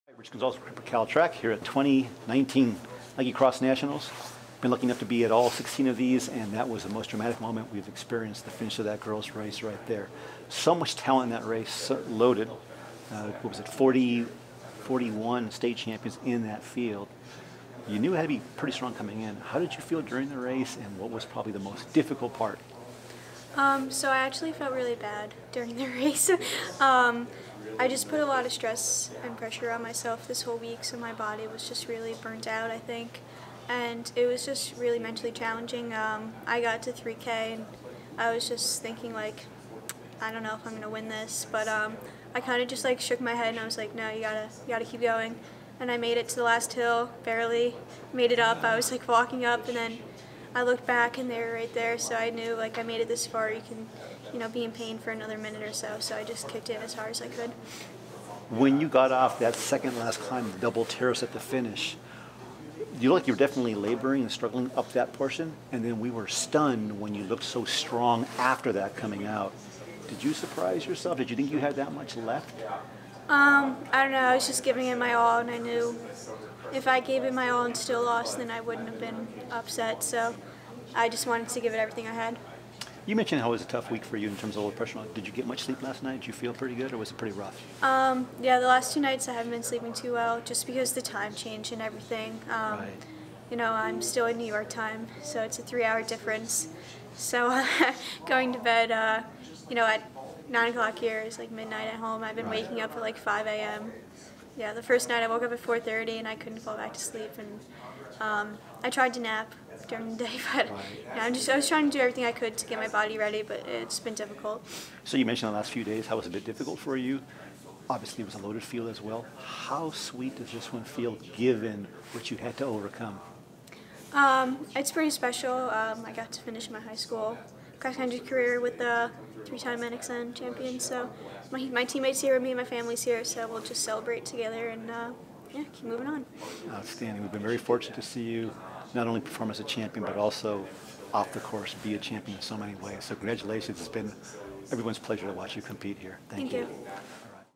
Katelyn Tuohy wins third consecutive Nike Cross Nationals title!
North Rockland HS of New York senior Katelyn Tuohy became the first high school-aged runner in history to win three Nike Cross Nationals individual titles, doing so in thrilling fashion. After seeing much of her lead dissipate over the final mile on the Glendoveer Course challenge, Tuohy found the strength to unleash one final burst of energy just as she was about to be caught by the chase pack. Her newfound momentum created a final surge to seal the historic victory! As you will hear, it was not an easy week for Katelyn leading up to the race. Here's her interview with Rich Gonzalez and Dylan Stewart of PrepCalTrack!